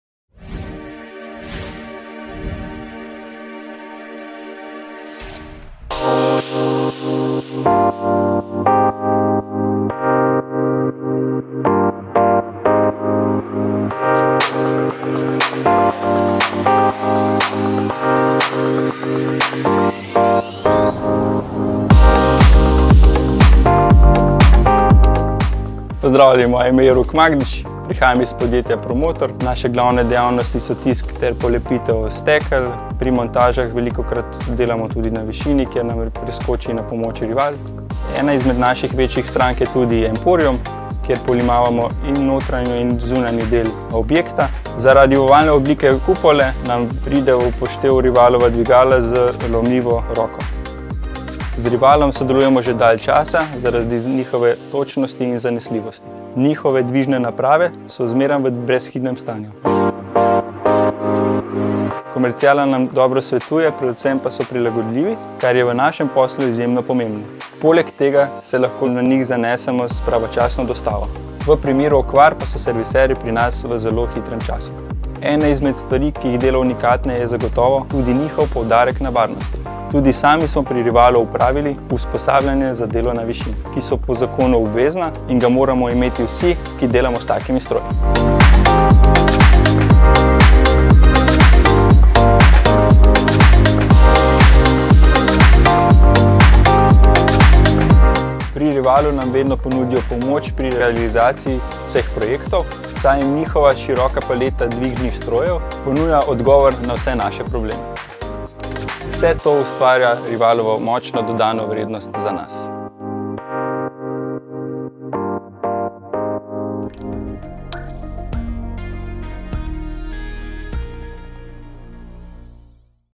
Se en af Riwals spændende projekter fra Slovenien
Med Riwals brede liftflåde, har du mulighed for, at løse enhver opgave i højden. I videoen får du indblik i, hvordan en Slovensk kunde bruger en knækarmsbomlift til at navigere omkring udfordrende vinkler.

Service er en af kerneværdierne hos Riwal. Som det kan ses i videoen, giver Riwal en skræddersyet service til kundens behov, herunder levering af de bedste maskiner og kyndig vejledning. Ligeledes har Riwal leveret IPAF-sikkerhedstræning til operatørerne, så sikkerheden og effektiviteten ved arbejde med liften kan nå nye højder.